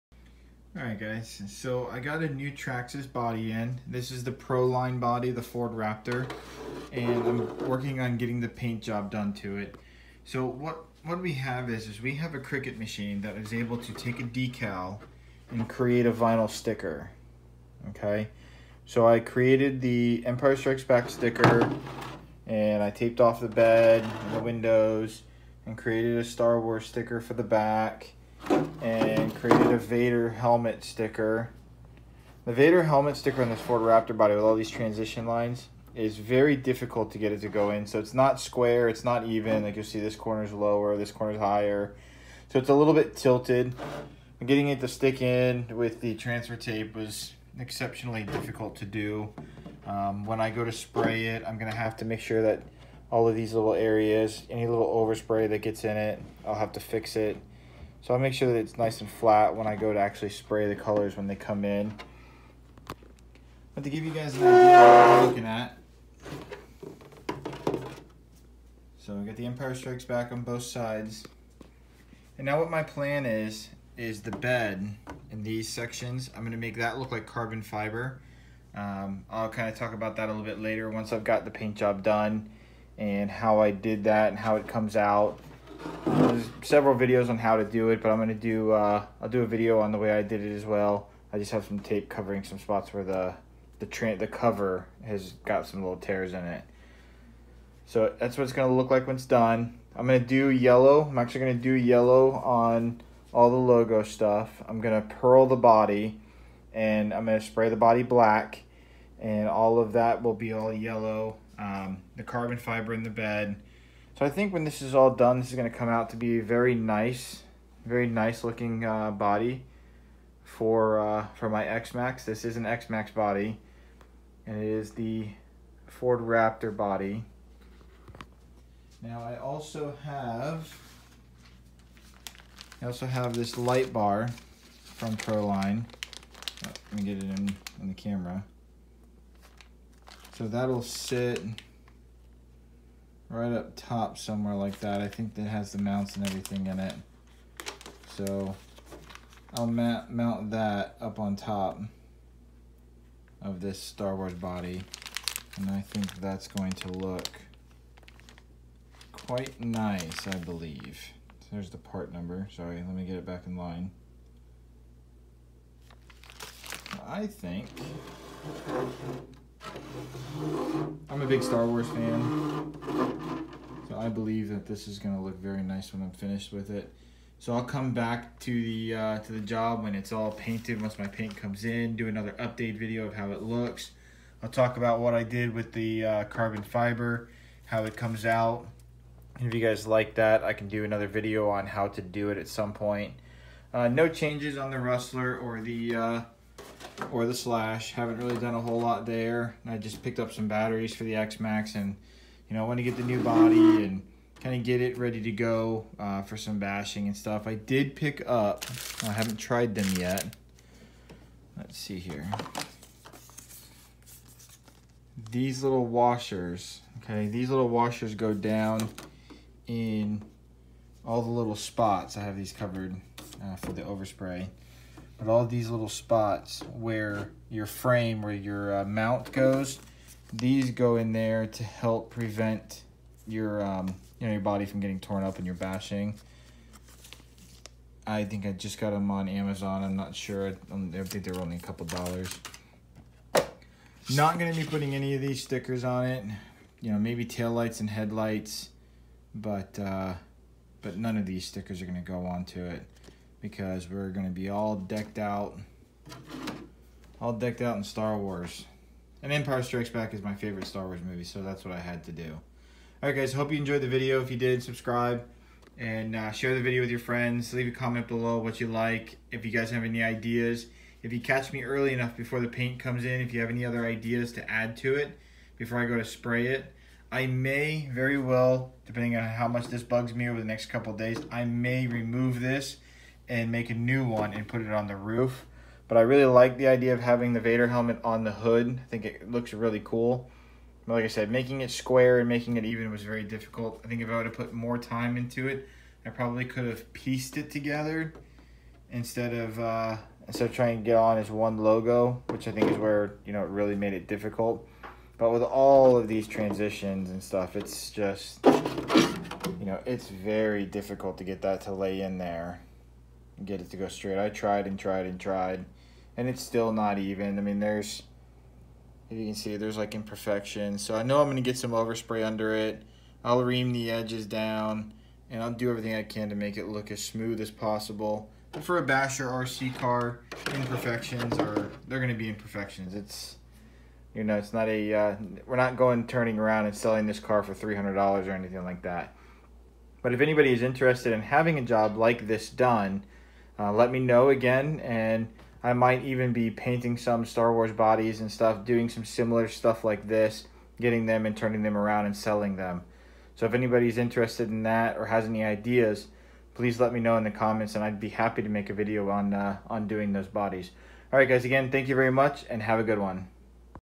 X maxx body painting. Star Wars theme
This is my proline ford raptor body. I’m painting it in a Star Wars. Empire Strikes Back theme.

As soon as my paint comes in I’ll record some videos of the progress so you guys can see how this project comes along.

For The words and emblems I used a Cricut machine to cut some automotive quality wrapping vinyl and stuck it to the inside of the body.

Sadly the Vader outline is not square on the hood but for A basher body it will work and look fantastic!

I also have the light bar for the roof that proline sells.